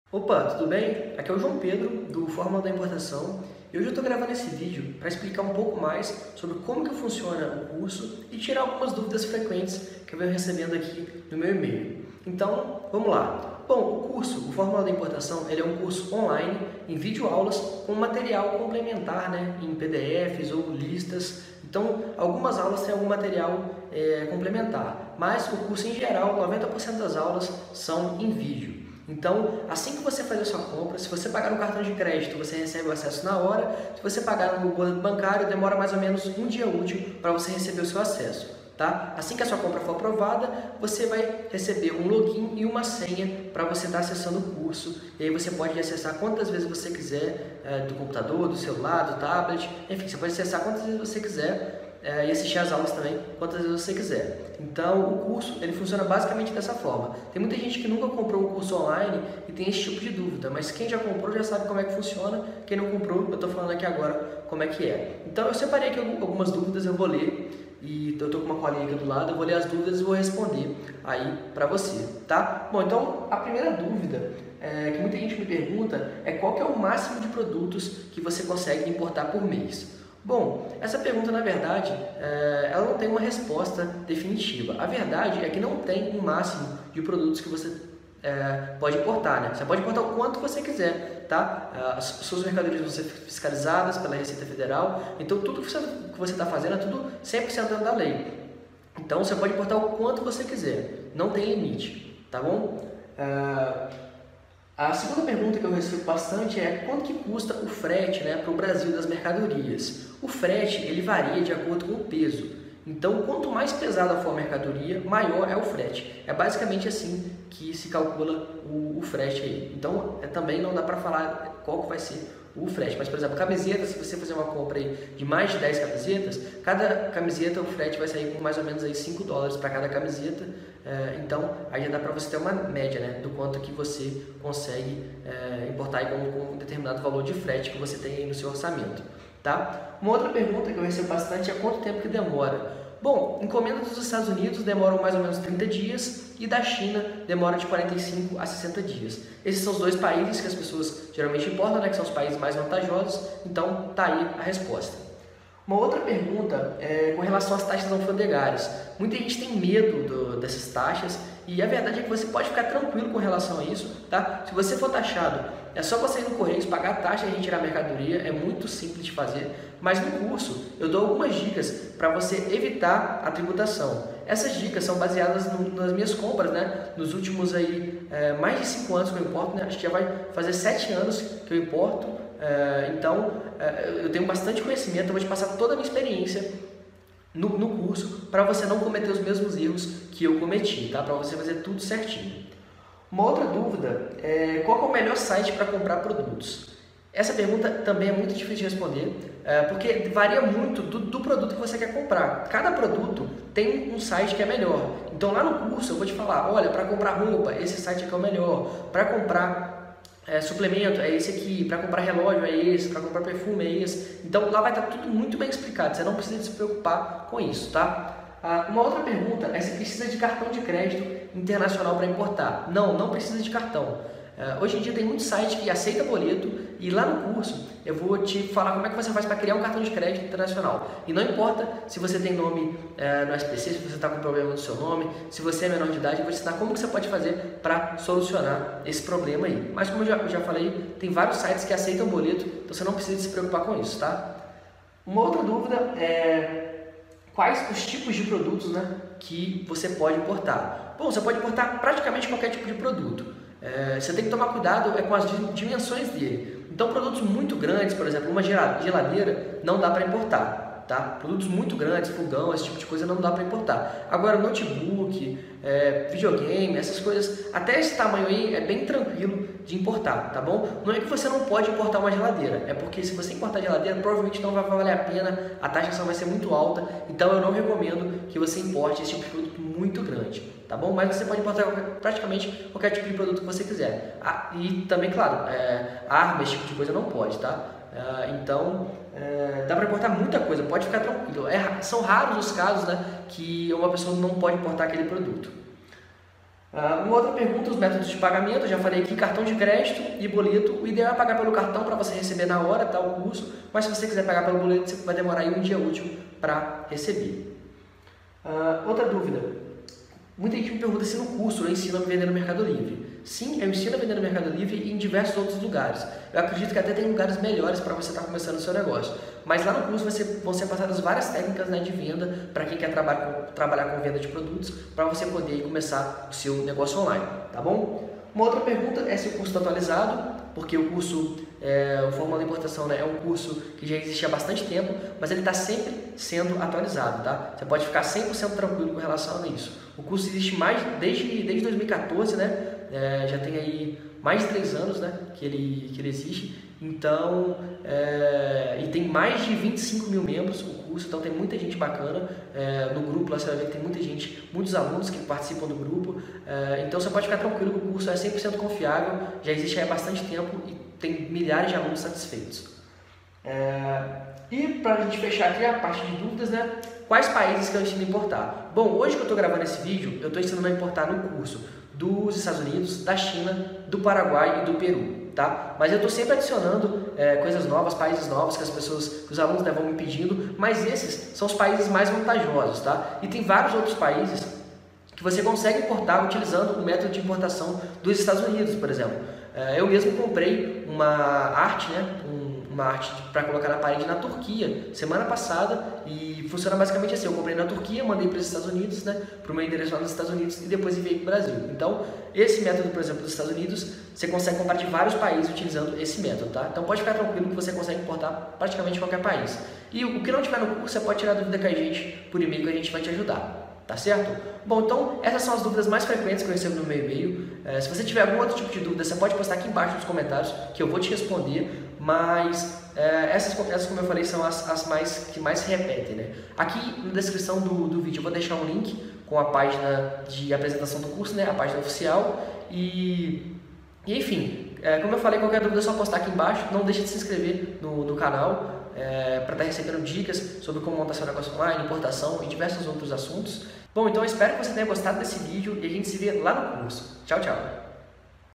Como Funciona o Curso Fórmula da Importação
Entenda como funciona o curso Fórmula da Importação.

Eu vou te mostrar toda a área de membros pra você entender como funciona o Fórmula da Importação, o melhor curso de importação da internet.